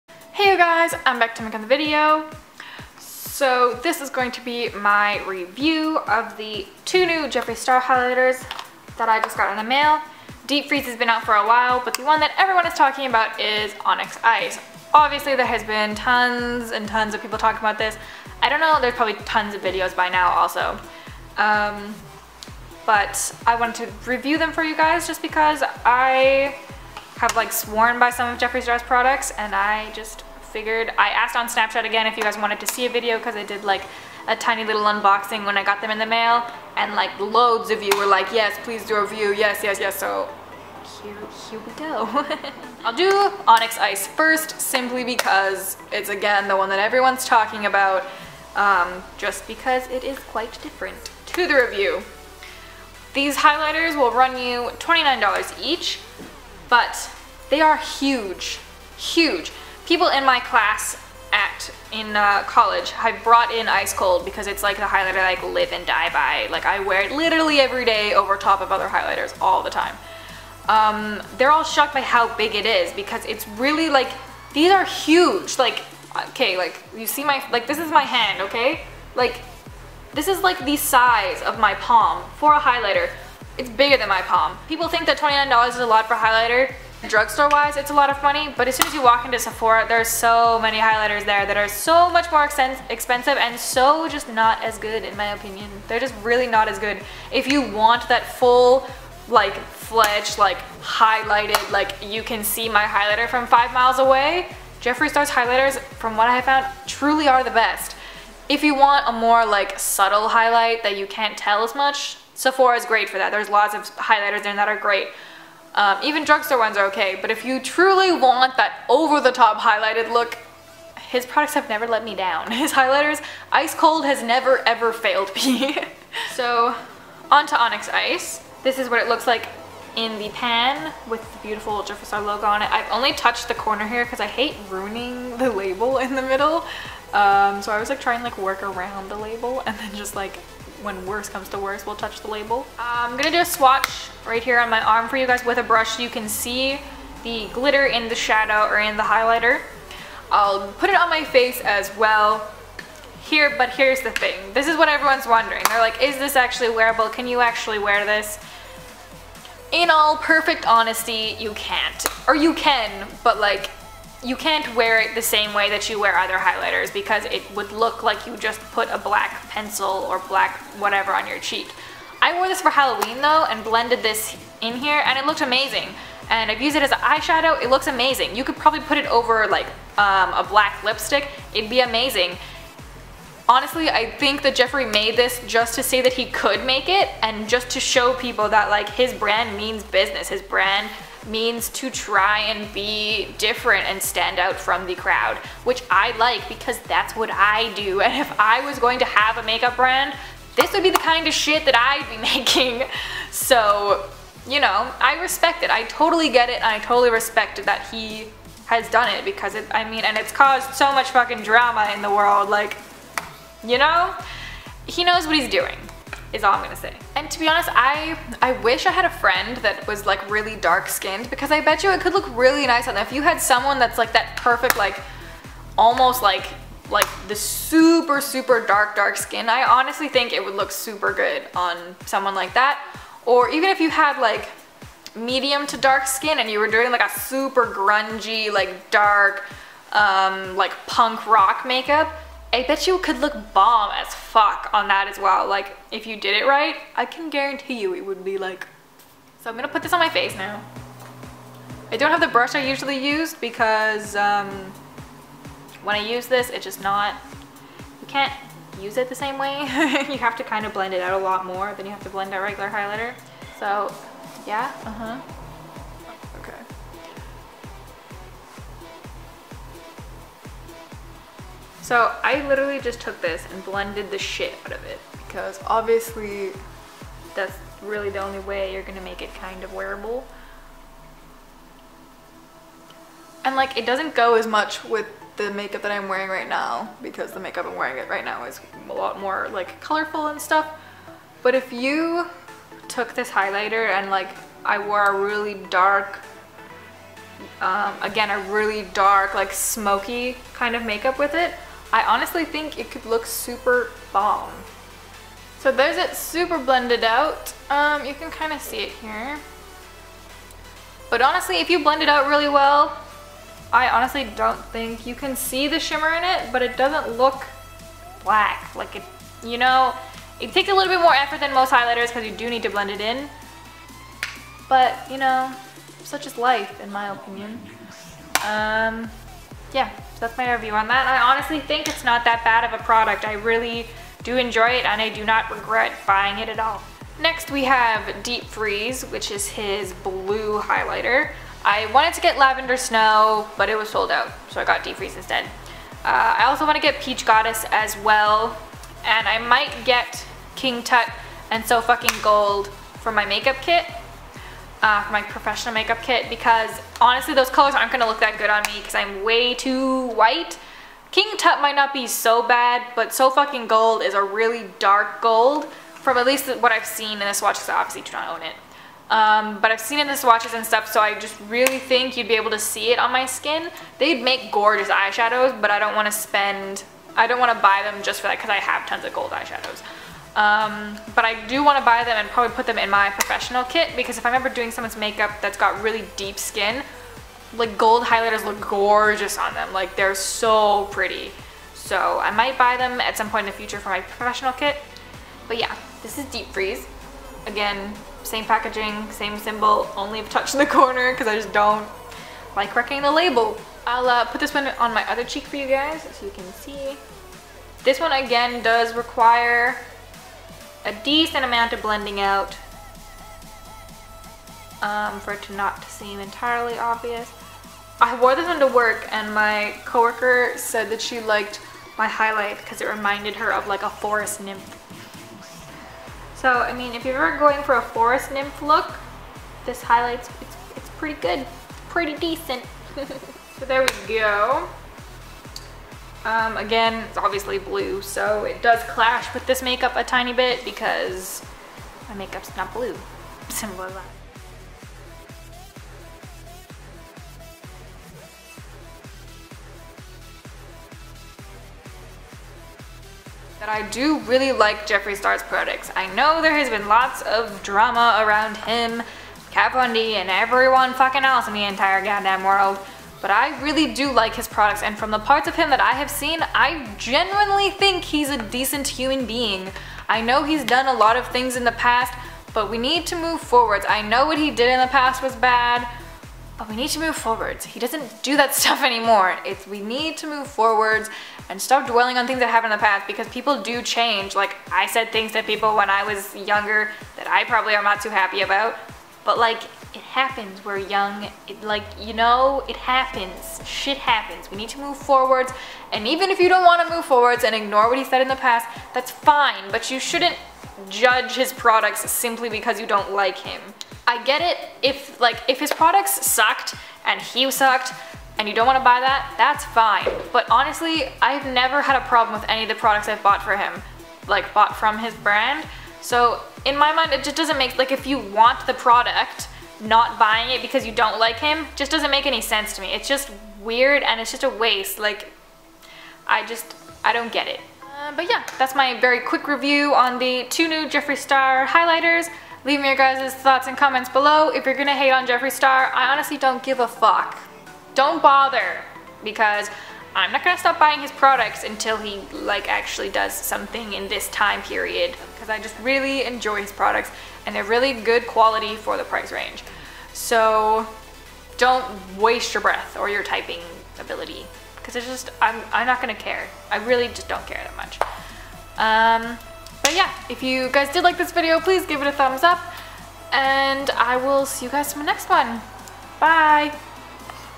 ONYX ICE and DEEP FREEZE by Jeffree Star Cosmetics Skin Frost Review, Swatch and Uses
Heyo guys! I hope you enjoy the video! The Kat Von D Metal Matte palette review will be up next week so be on the look out for that! :)

Links in case you want to creep me in my everyday life ;)